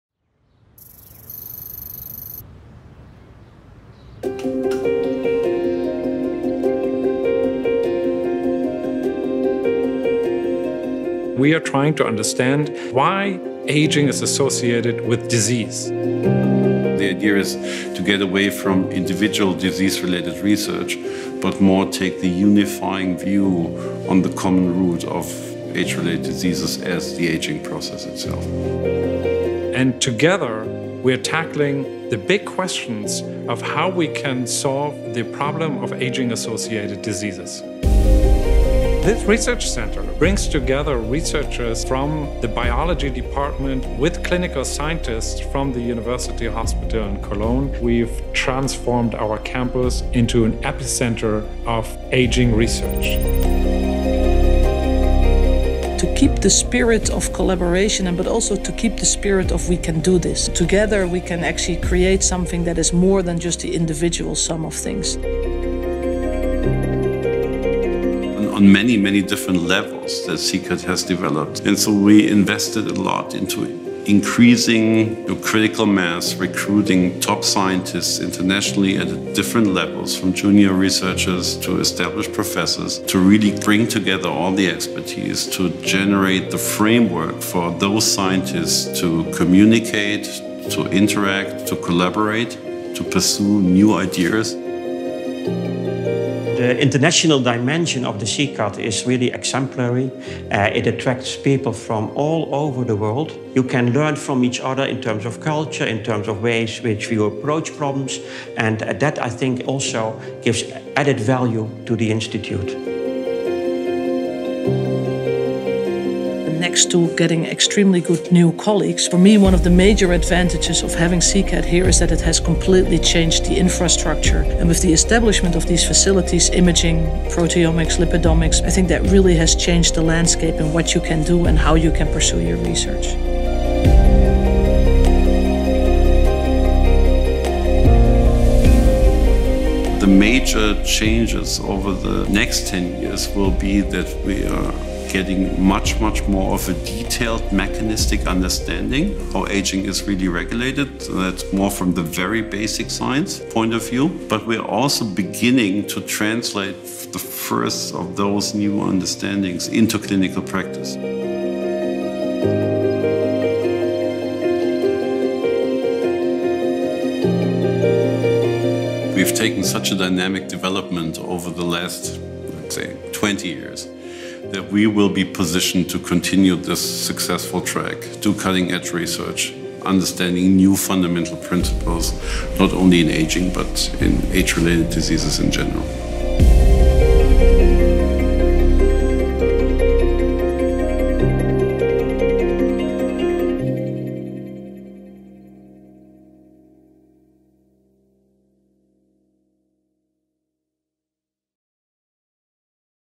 CECAD - Excellent in Aging Research
The Cologne Cluster of Excellence in Cellular Stress Responses in Aging-associated Diseases provides a dynamic environment for research into the aging process itself and related diseases. Scientists from all over the world tackle the common basis of diseases like cancer, neurodegenerative diseases or type 2 diabetes.